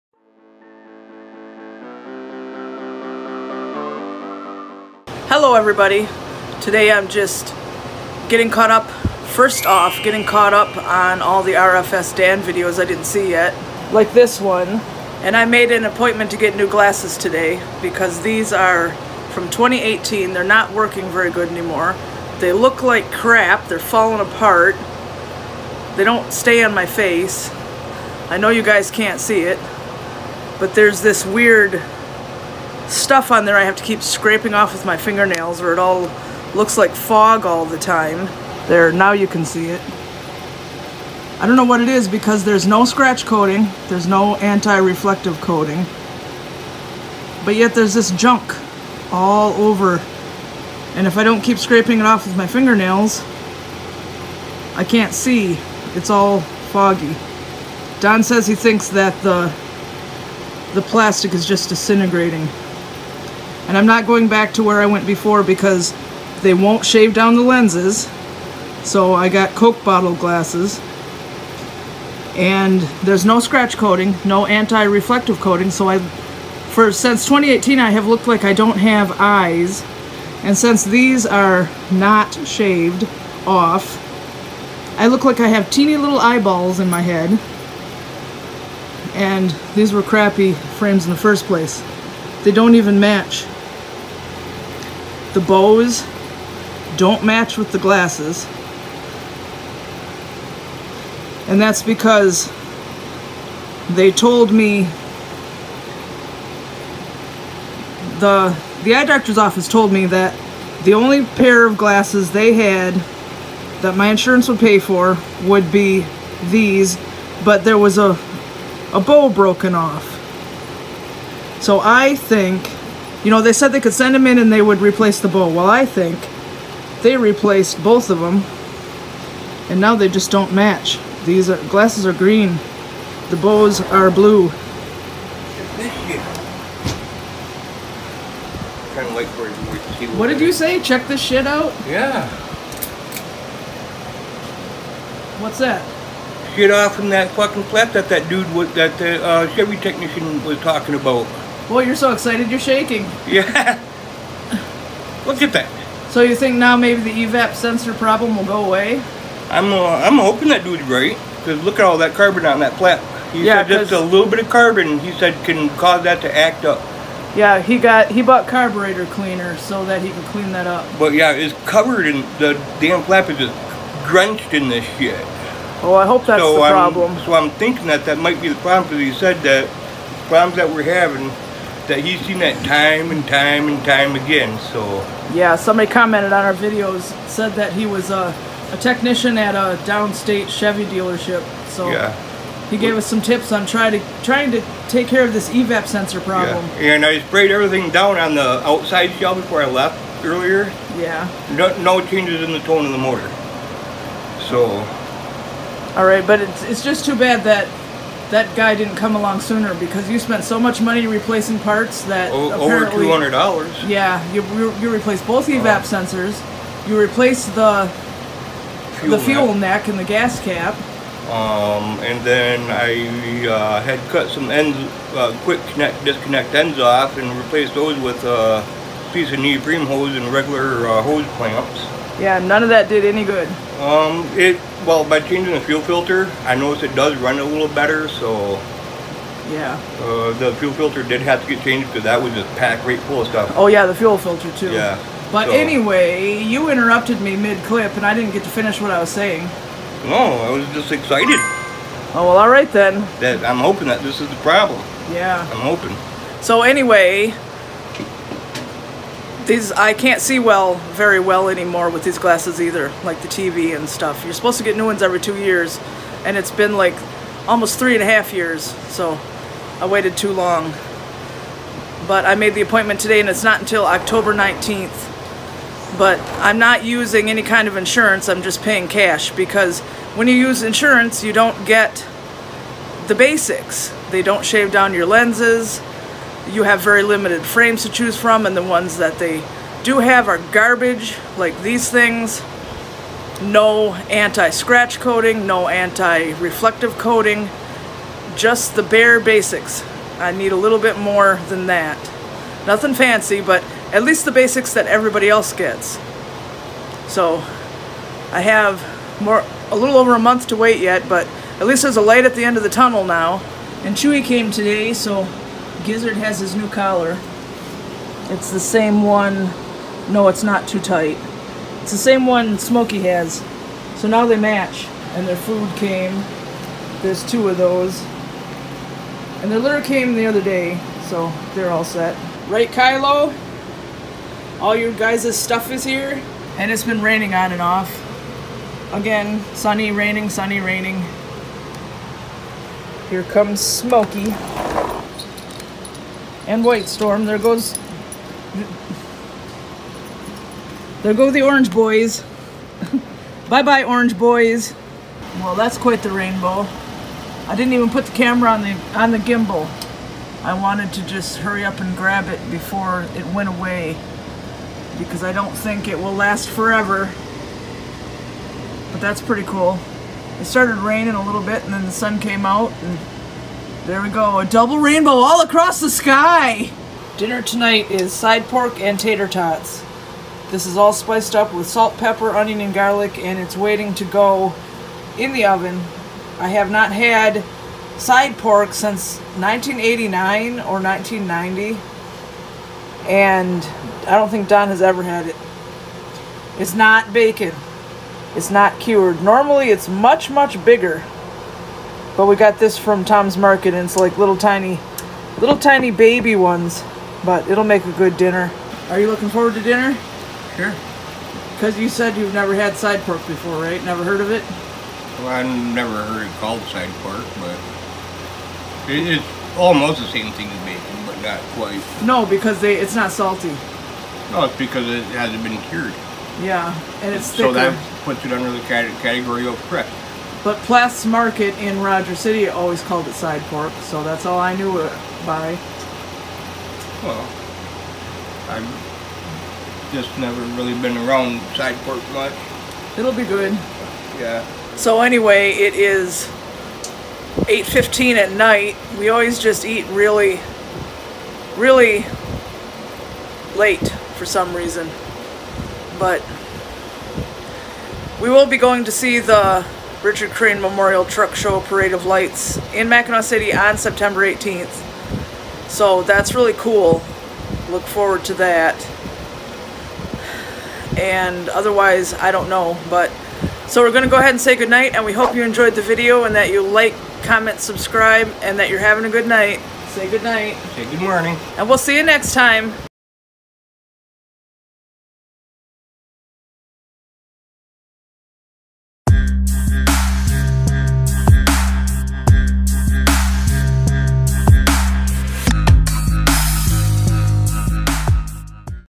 My Glasses Are Garbage!
I'm finally going to get new glasses, which is really good because my glasses are garbage!  Also, Gizzard gets his new collar from Chewy and Don works on the Trailblazer some more to try to locate the evap problem!

Instagram: @WiltonInRealLife @SmokeyandtheGizzard @whitestorm_warrior @tennille_wilton_art @kylo_tabby_cat @graystripe_warrior_cat @spottedleaf_medicine_cat_ @magnetfishingdeerman

StoryFire: @Wilton In Real Life